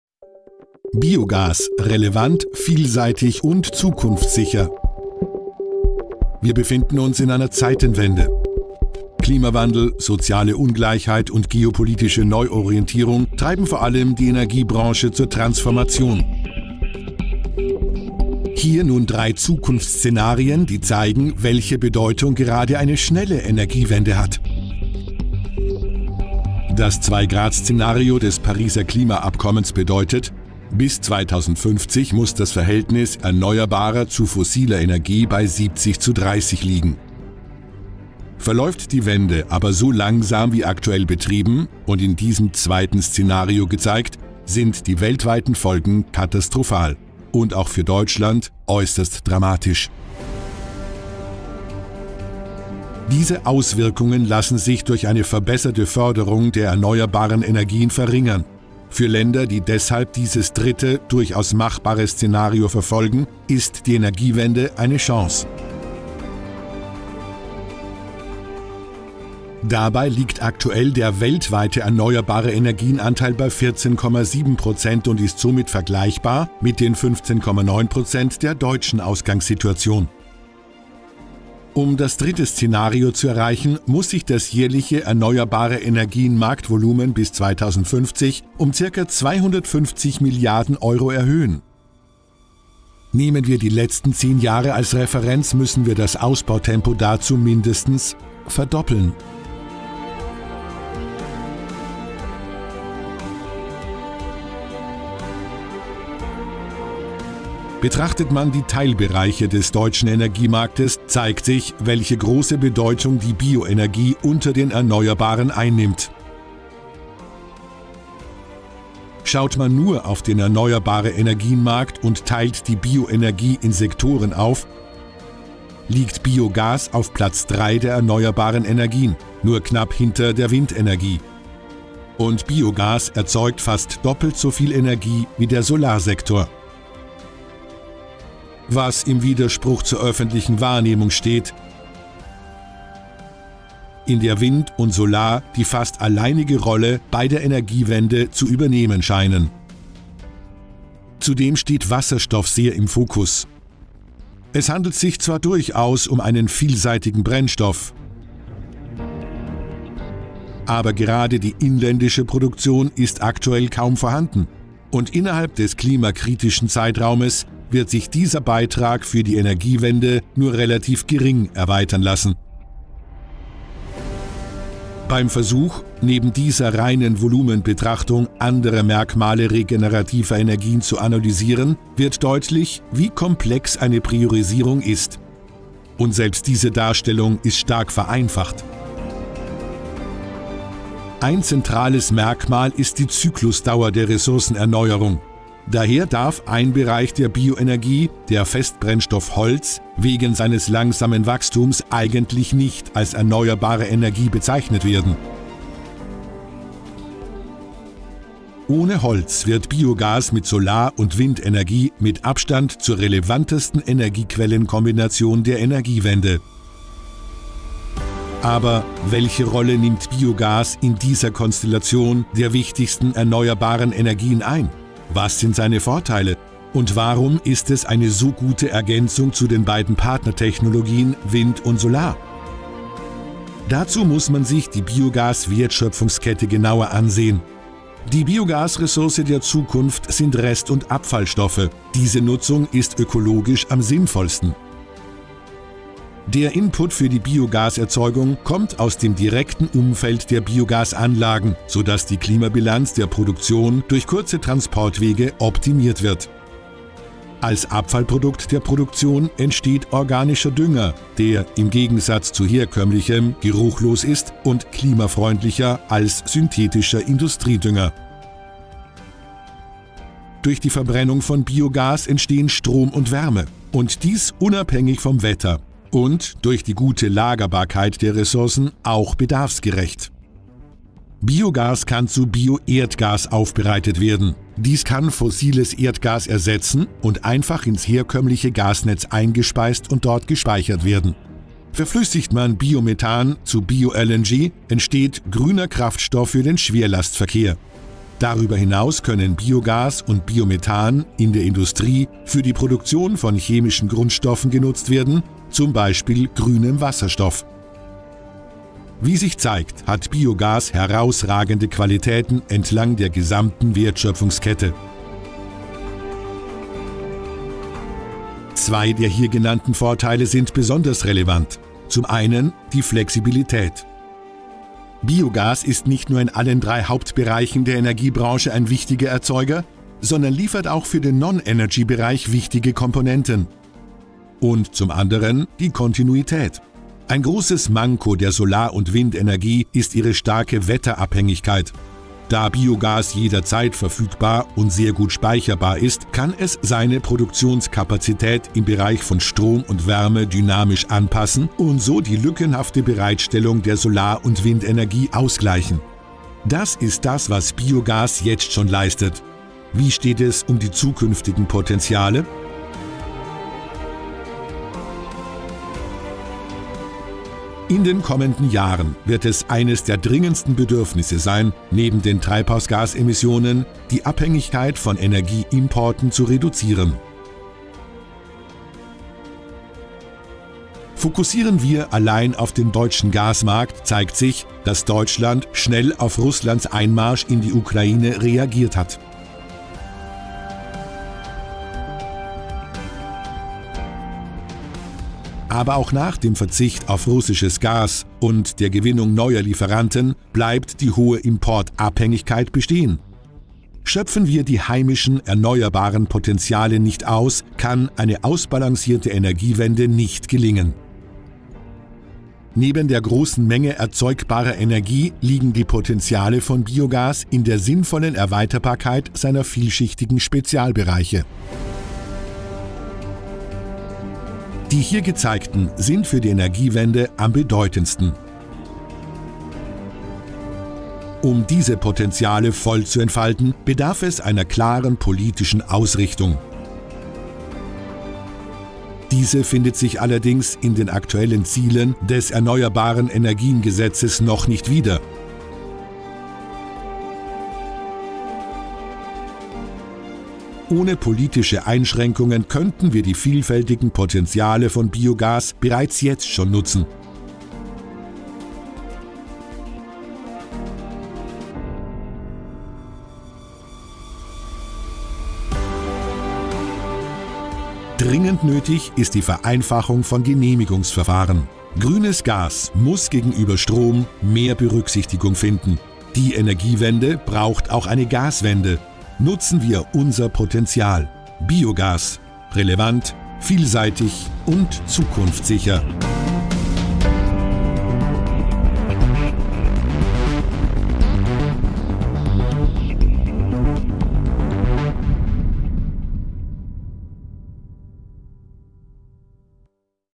Biogas: Relevant, Vielseitig, Zukunftssicher
Relevant, vielseitig und zukunftssicher – mit diesen drei Eigenschaften überzeugt Biogas im Erneuerbaren Energien-Mix und wir als ebenso flexibles wie innovatives Unternehmen und sehen uns als Motor der Energiewende. Aktuell befinden wir uns in einer Zeitenwende und müssen die Transformation in der Energiebranche schneller und noch effektiver vorantreiben, um den jetzt schon spürbaren Folgen des Klimawandels die Stirn zu bieten. Unser Kurzfilm liefert Daten, Zahlen und Fakten rund um das Multitalent Biogas als Gamechanger für die Energiewende!

Unser Kurzfilm liefert prägnante Daten, Zahlen und Fakten rund um das Multitalent Biogas! Alle Aussagen basieren auf aktuellen Statistiken und Artikeln des unabhängigen Denk- und Politiklabors AGORA Energiewende, Statista, Jahresbericht AGEB 2022, dena-Analysen u.a. Die Zusammenschau erhebt dabei keinen Anspruch auf Vollständigkeit.

Ein komprimiertes Quellenverzeichnis ist auf Anfrage verfügbar.

Biogas: Relevant. Versatile. Future-proof.

Relevant, versatile and future-proof - with these three characteristics, biogas is convincing in the renewable energy mix and we as a flexible and innovative company see ourselves as a motor of the energy transition. We are currently at a turning point and need to drive the transformation in the energy sector faster and even more effectively in order to counter the already noticeable consequences of climate change. Our short film provides data, facts and figures about the multi-talent biogas as a game changer for the energy transition!

Our short film provides concise data, facts and figures on the multi-talent biogas! All statements are based on current statistics and articles from the independent think tank and policy laboratory AGORA Energiewende, Statista, the AGEB 2022 annual report, dena analyses, etc. The synopsis does not claim to be complete.

A condensed list of sources is available on request.

0:00 Einleitung in das Thema Biogas
0:18 Die Energiewende
1:43 Erneuerbare Energien
3:41 Was ist Biogas?
6:04 Große Chancen müssen genutzt werden
7:50 Zusammenfassung / Fazit Biogas